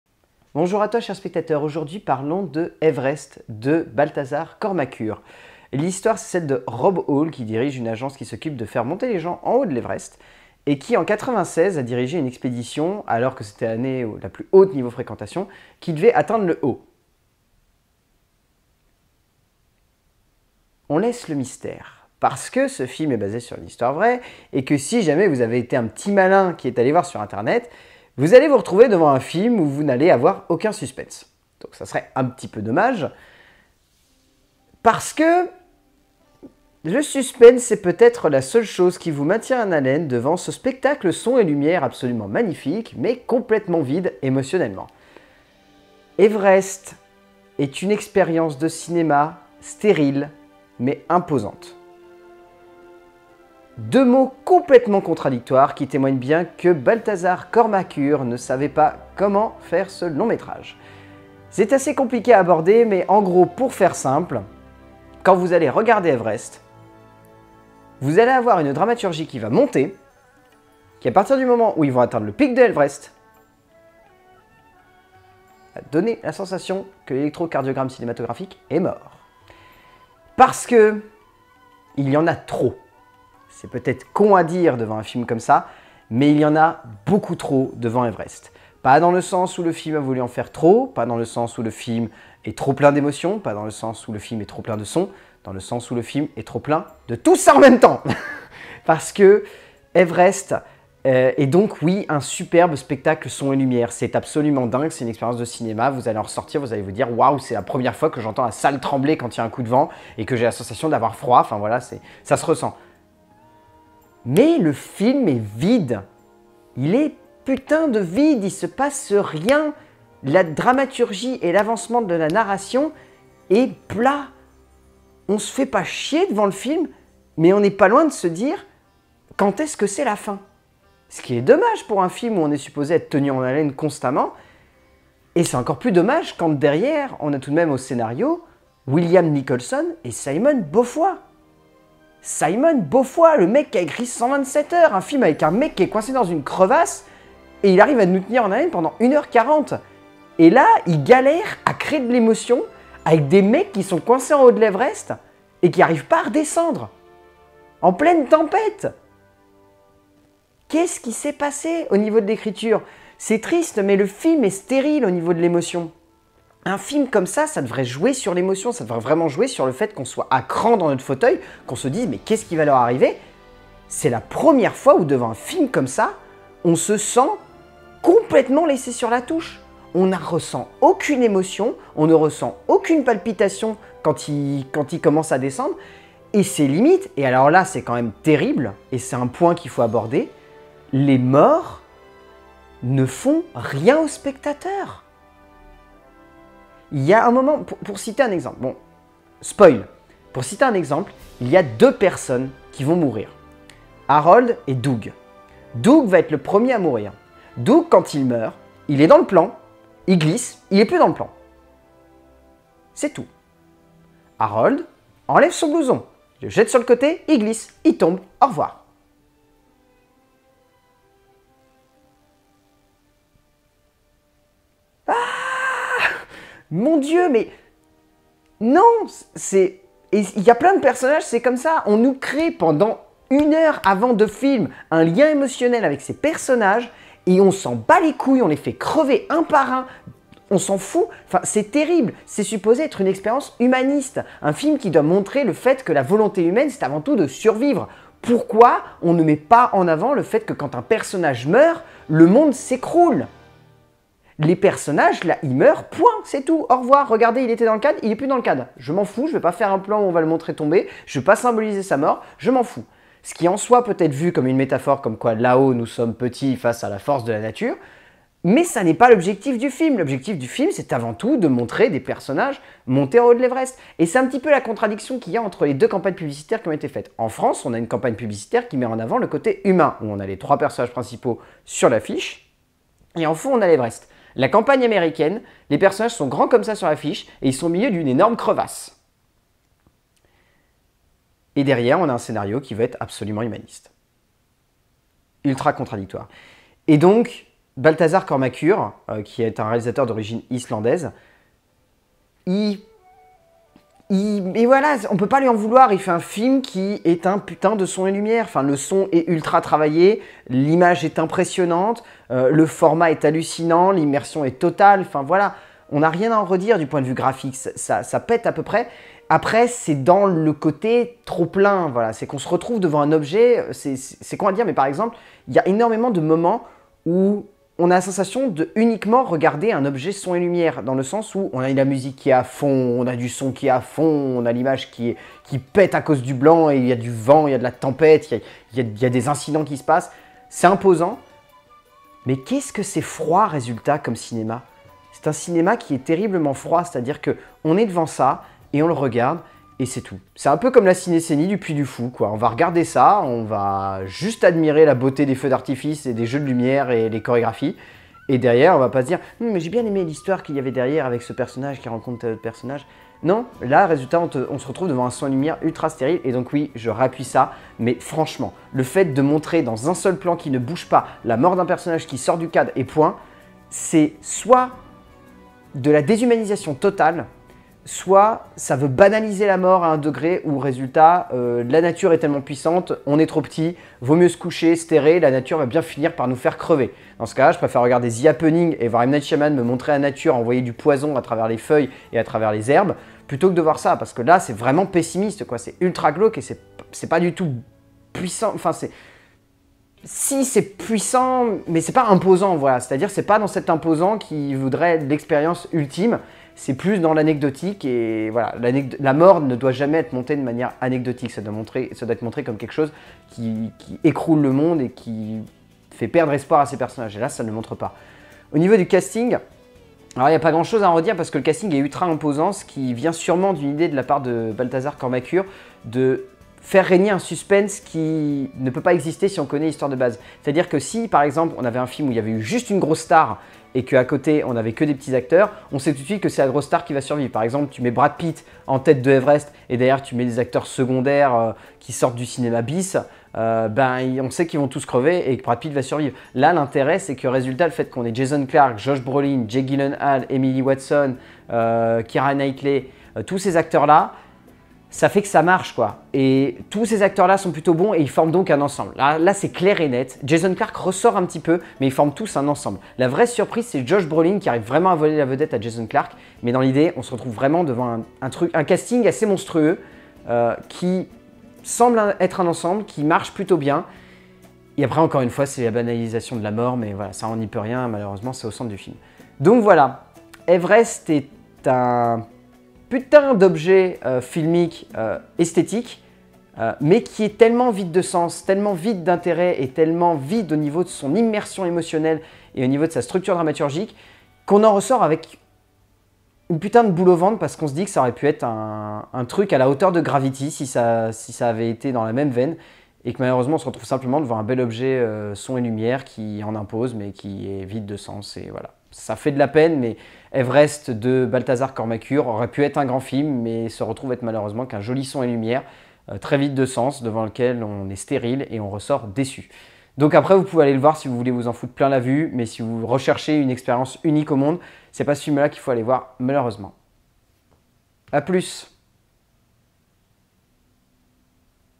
LCDC - Everest
Vlog critique sur la sortie d'un sacré film, une oeuvre à mi chemin entre la cinéscénie du puy du fou, et un film tellement plat que l'on ne sait pas quoi en tirer à la fin, "Everest"